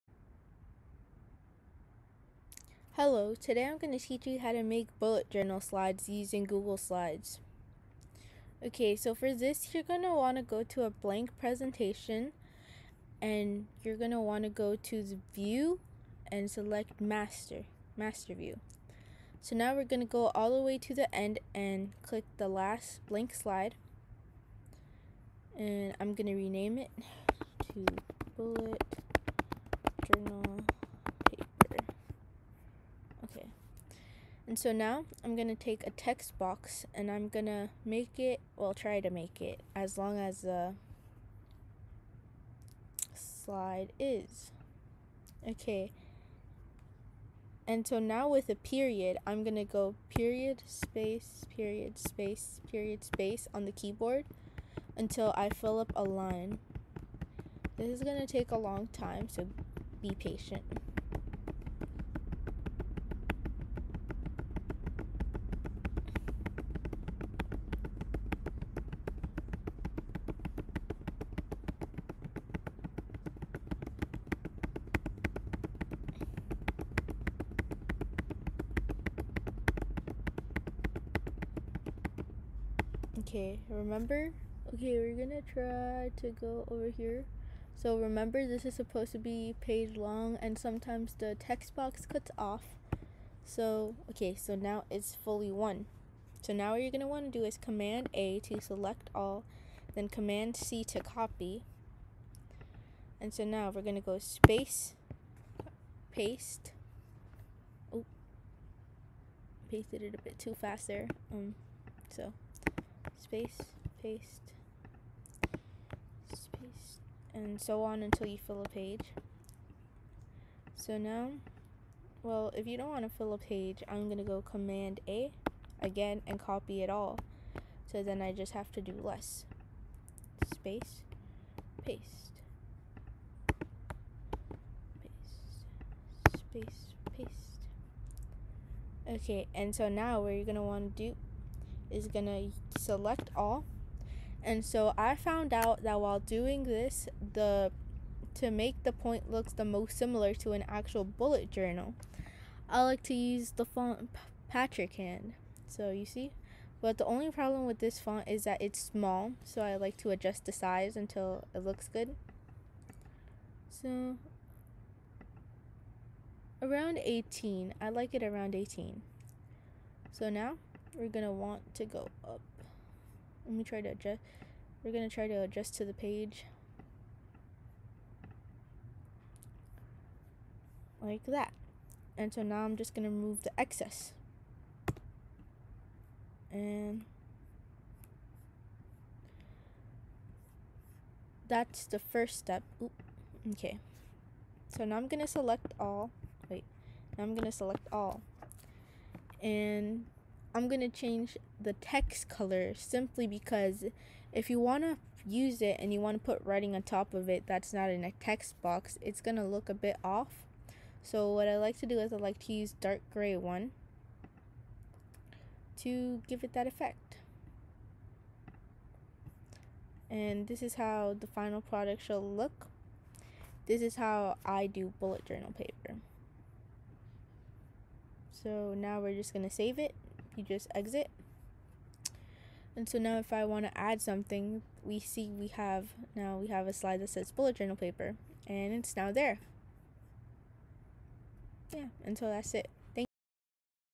Dotted Paper (Bullet journal paper) tutorial on google slides!
Hello
Today I'm going to teach you how to make a dotted grid also known as a bullet journal paper template on google slides. Hope you guys find this video helpful.